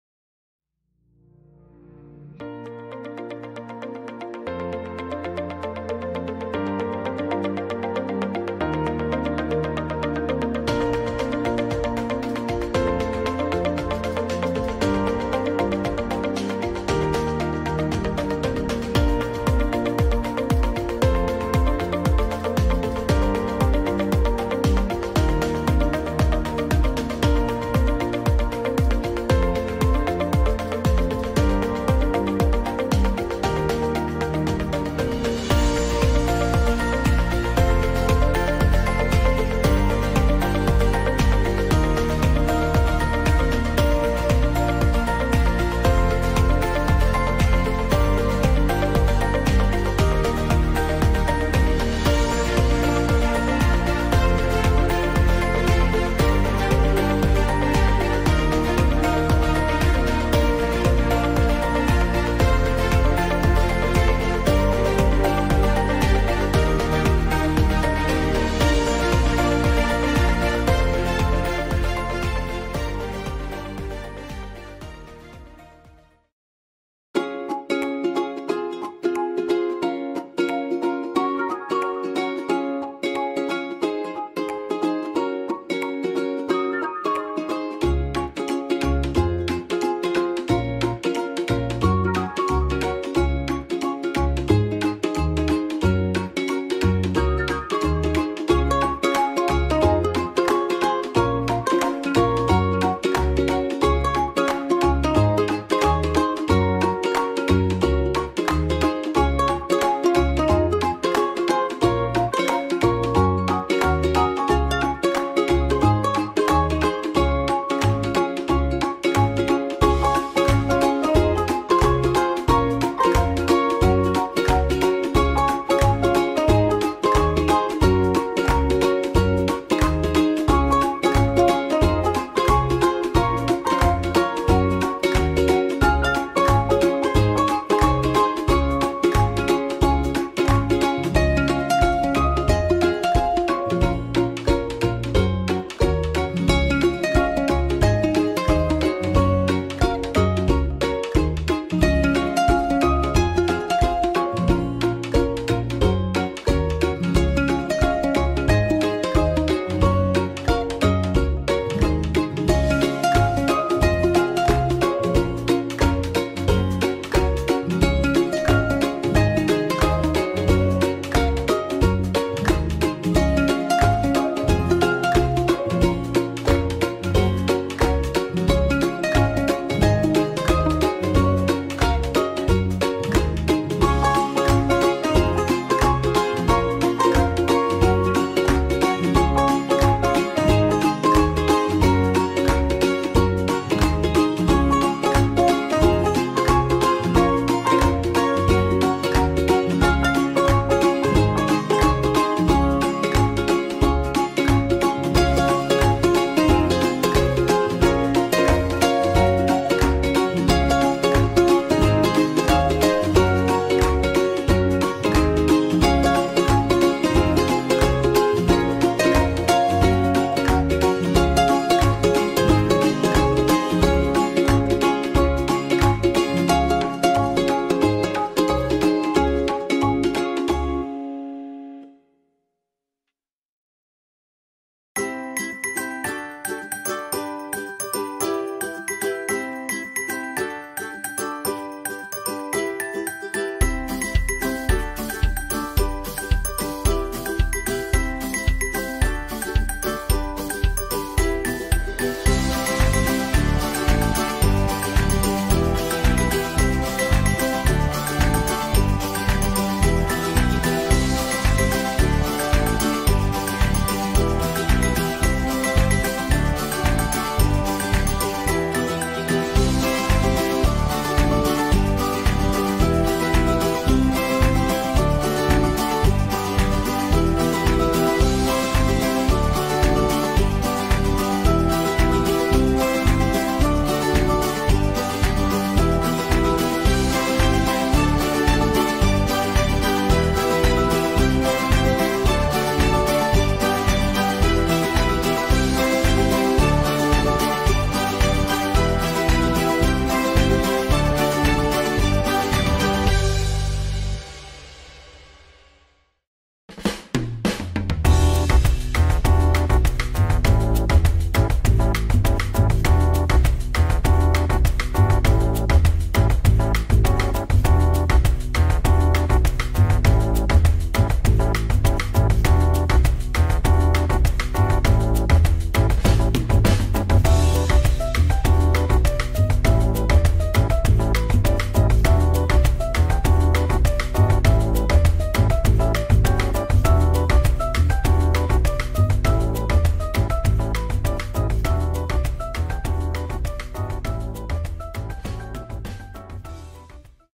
Garlic bread n cheese dip - Taste that linger & asks for more :)
Garlic bread n cheese dip are made from most of the ingredients available at  stores near you. Make restaurant like Cheese dip & Garlic bread for your loved ones.